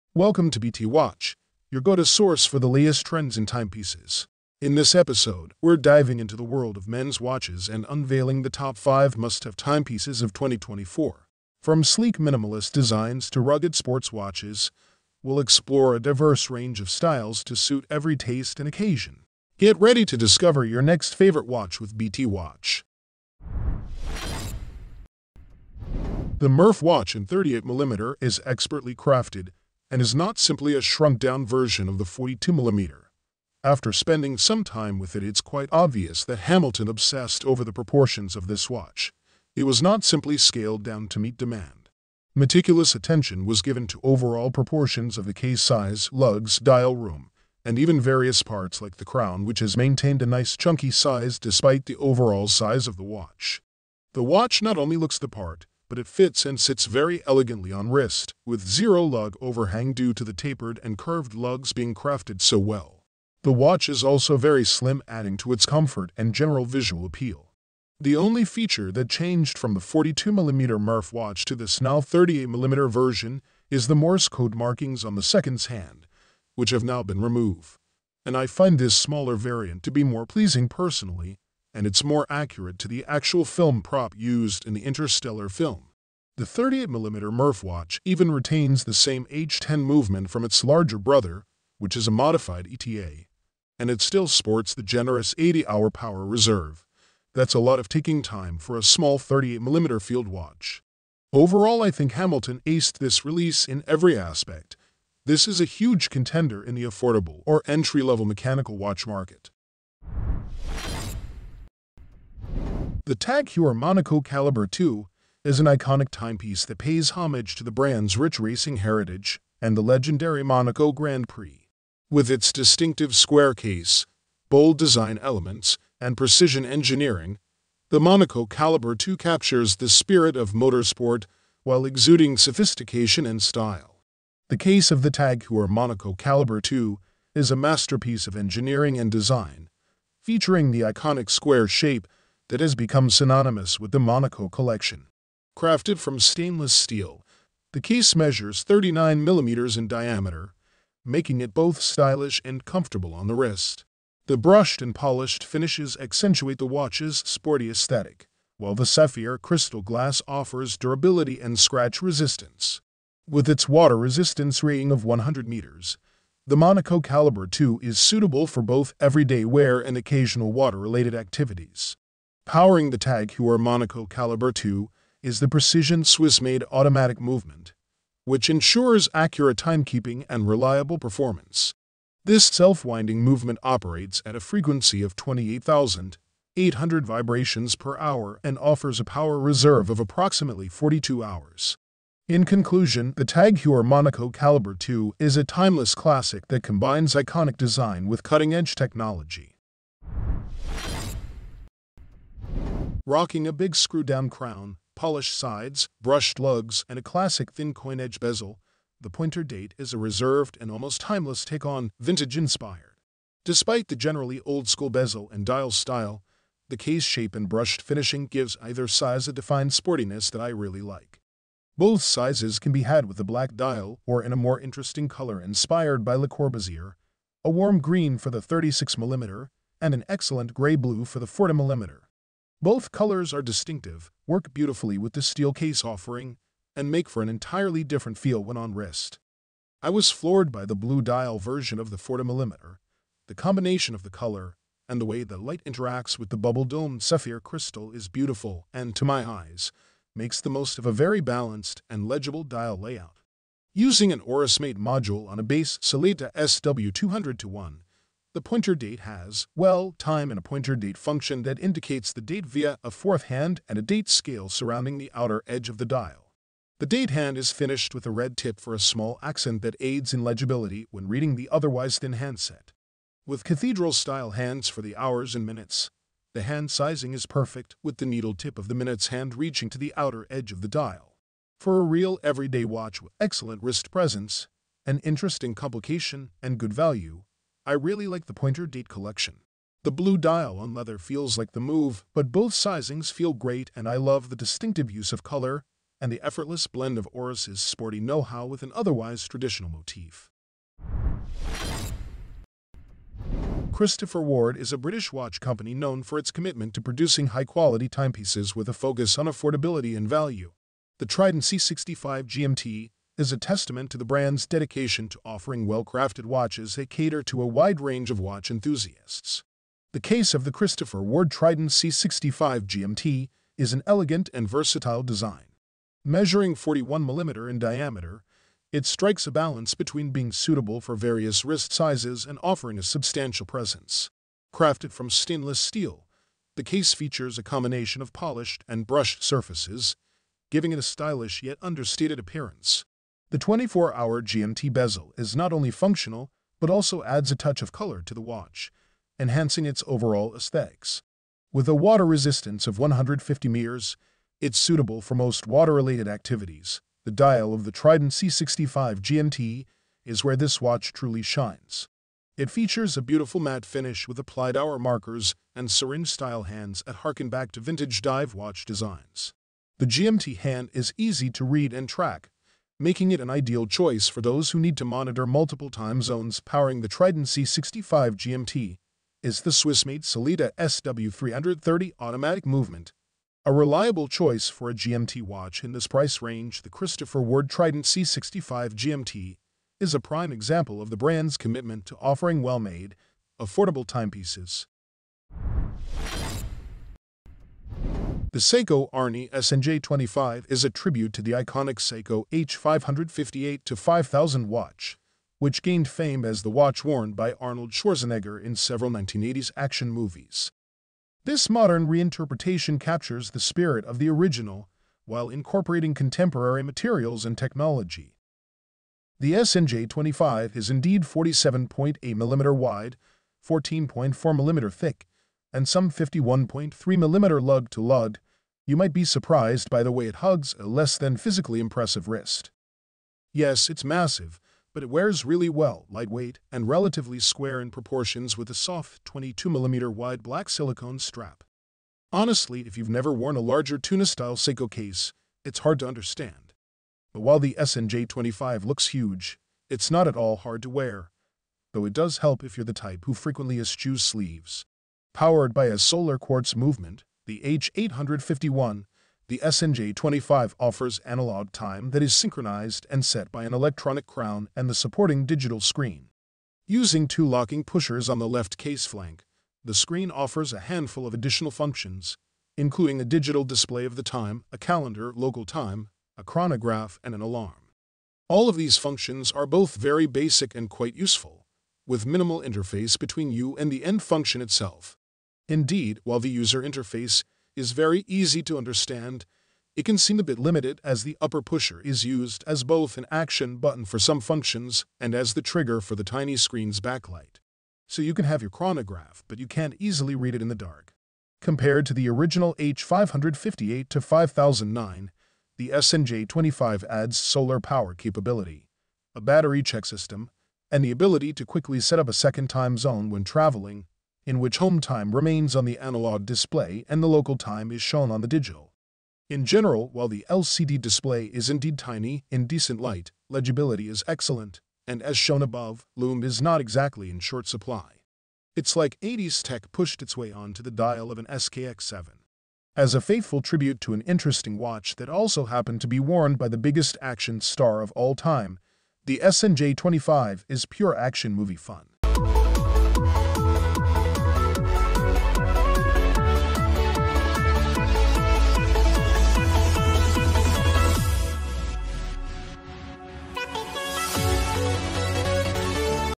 Top 5: Best Watches For Men To Buy In 2024!
⌚ 1. 00:26 Hamilton Khaki Field 38mm
⌚Similar Products Aliexpress
⌚ 2. 01:53 Tag Heuer Monaco Calibre 2
⌚ Similar Products Aliexpress
⌚ 3. 03:23 Oris Big Crown Pointer Date
⌚ Similar Products Aliexpress
⌚ 4. 05:03 Christopher Ward Trident C65 GMT
⌚ Similar Products Aliexpress
⌚ 5. 06:38  Seiko Arnie SNJ025